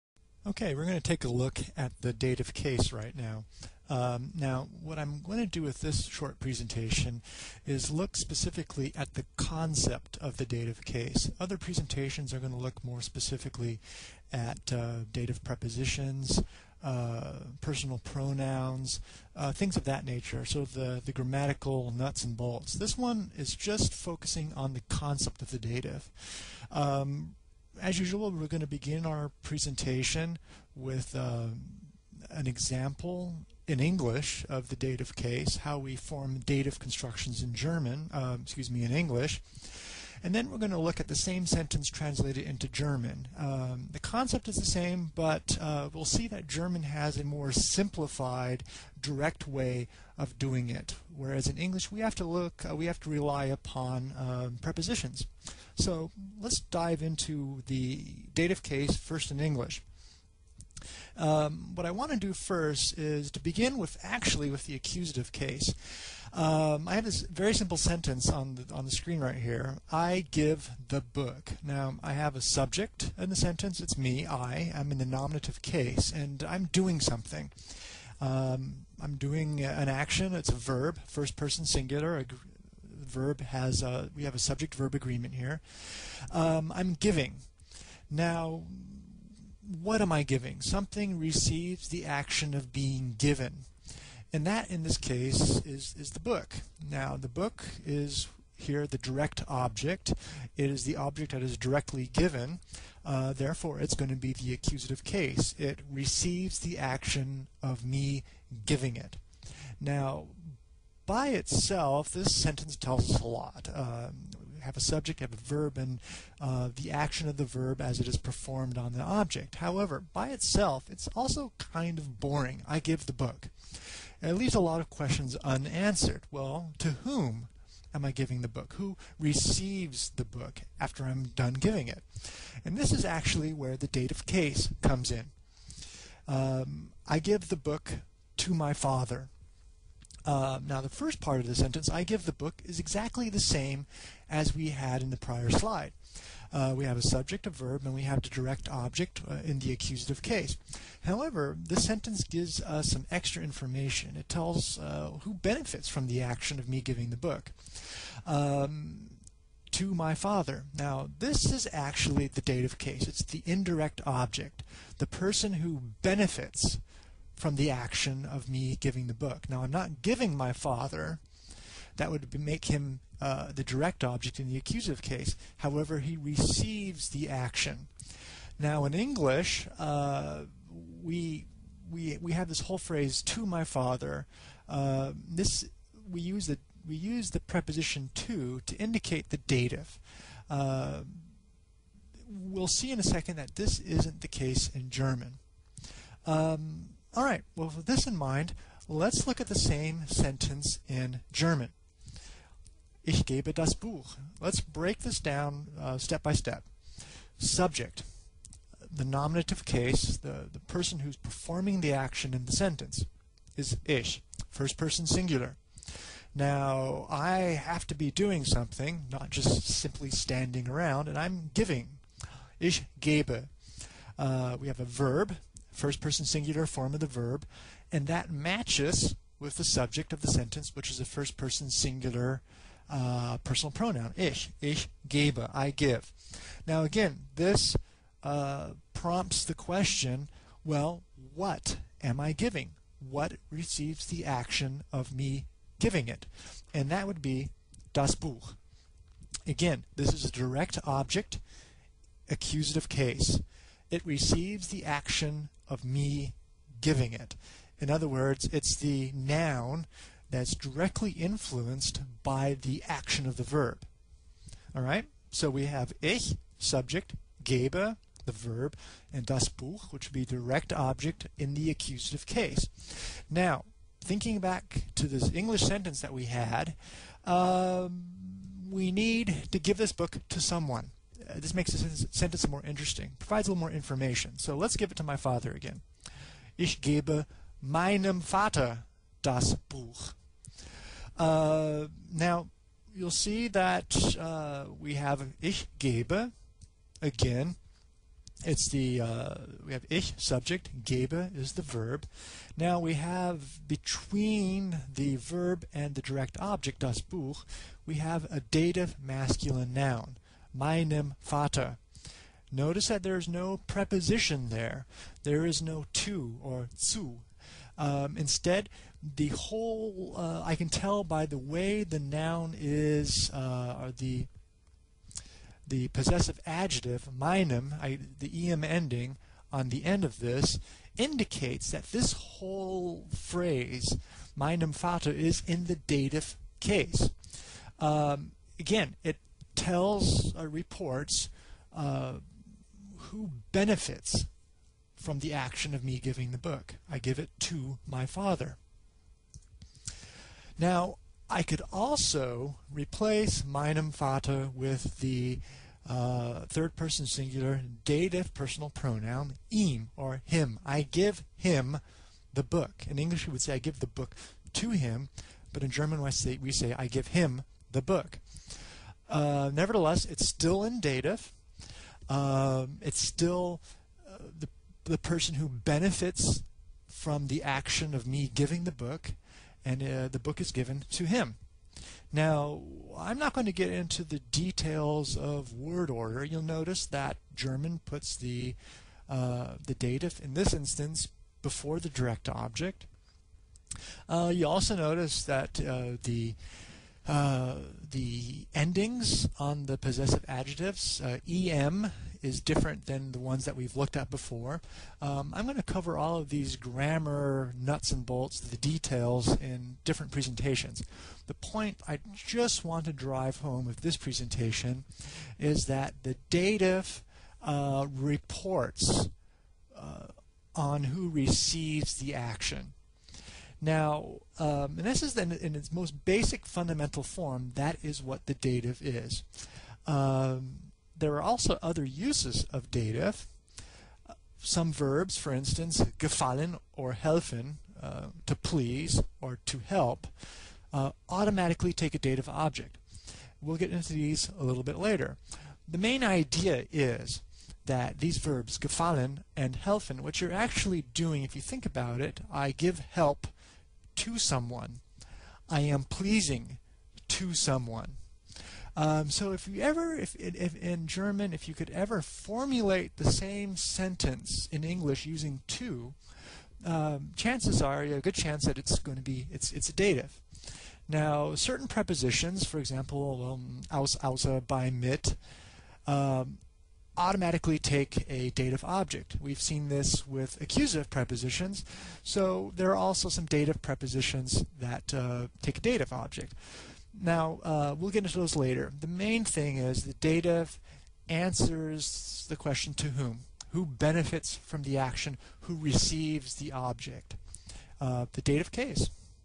The Dative Case in German
Video of class lecture slides introducing the concept of the dative case in German. The video begins first with an examination of the dative case in English before moving into a discussion of the dative in German. The video then demonstrates how a dative object can be substituted with a dative personal pronouns before concluding with a brief overview of other times when the dative is used in German.

The video covers:

01:01 The dative case in English
03:25 Example of dative in German
05:55 Example with dative personal pronoun
07:21 Other instances when the dative is used